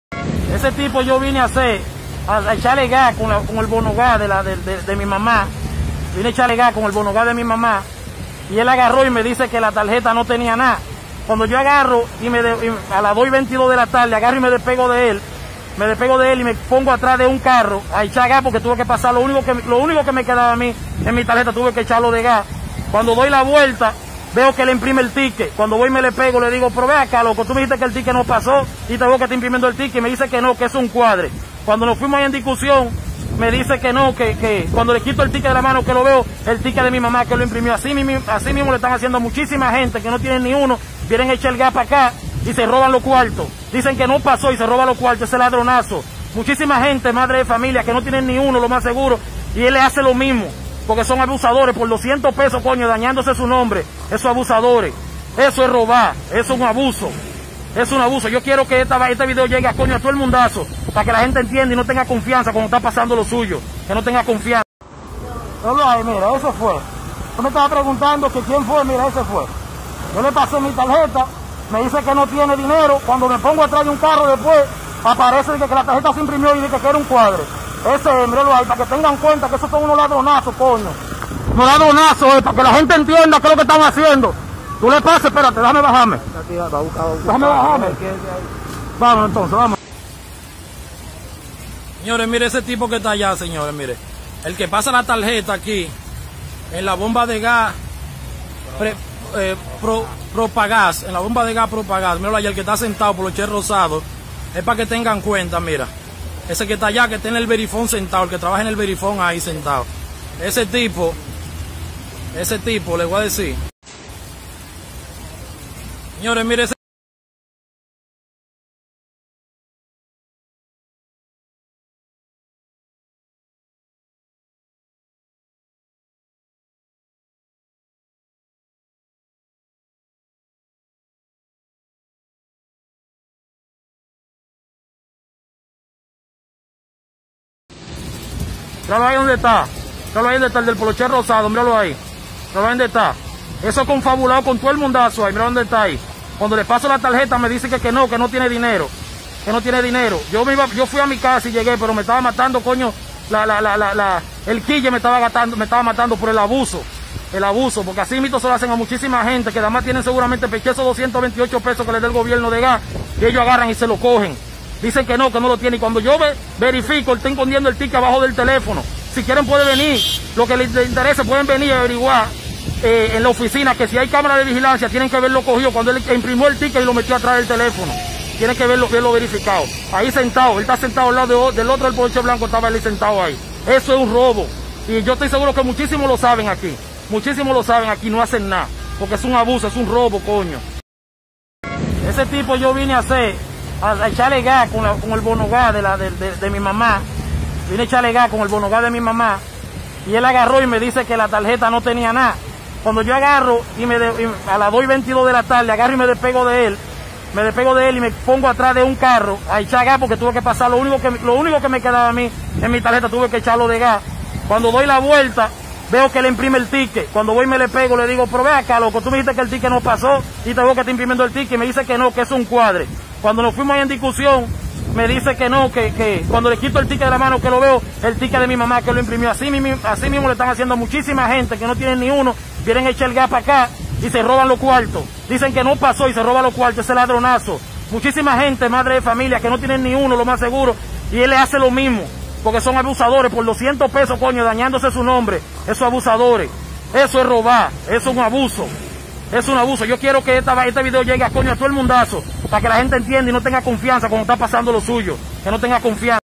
Estafas de Tarjeta solidaridad y Quédate en casa, bombero robó dos galones de gas a una familia
Continúan las estafas a beneficiados de Bono Gas, Solidaridad, Quédate en casa y comer es primero .

Este bombero le robó los dos galones de gas que le da el gobierno a una familia para cocinar durante un mes. 😓

La Romana.- Un joven muy  indignado acusa a un bombero de Propagas Romana de intentar engañarlo para quedarse con el dinero de la tarjeta Bono gas, Resulta que el joven en la tarde de hoy fue a la estación de GLP a llenar el tanque de su  madre, el bombero pasó la tarjeta y le dijo que no tenía nada tras retirarse se percata que el bombero imprime el ticket con la cantidad aprobada y lo esconde detrás de su celular a lo que el joven se le acercó le reclamó y le arrebató el ticket, el individuo lo negó pero luego le pidió disculpa por la acción.

"yo actué de esa manera  porque como me lo hizo a mí no se sabe a cuantas personas más ha engañado", exhorta a la población que tengan cuidado a la hora de ir a esa estación abastecerse, también le hace un llamado a la administración de Propagas para que intervengan en esta situación que observen los videos de las cámaras de vigilancia para que vean la acción de ese bombero y hagan los correctivos de lugar.

Power Santana Te Informa.